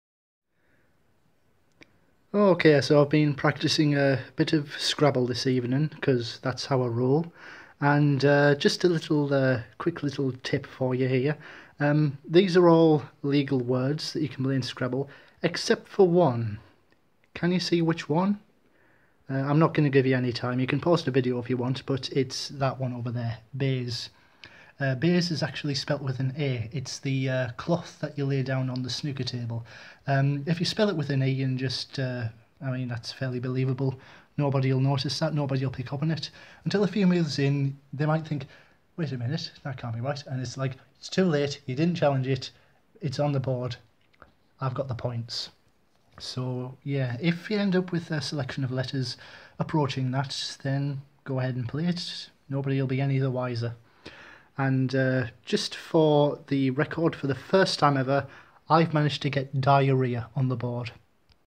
Hint & Tip - Scrabble (English UK)
In today's edition of Hint & Tip, I offer a sneaky little tactic you can try at home if you're needing rid of a few precarious letters. Additionally, when I said there was only one unacceptable word on the board, I lied; there are three. See if you can guess the other two. The prize is a fleeting sense of superiority.